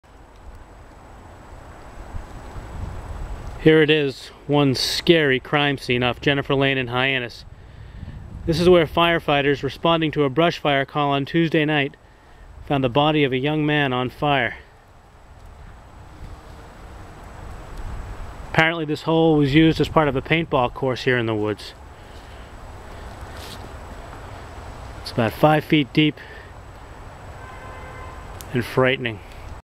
Murder crime scene in Hyannis woods
Cape Cod Times online reporter Eric Williams reports from this dirt hole in Hyannis, where the burned body of a local teen was found in the woods off Jennifer Lane in Hyannis, on Tuesday night 12/16/08. Friends and family have identified the dead youth as Jordan Mendes, 16, a student at Barnstable High School.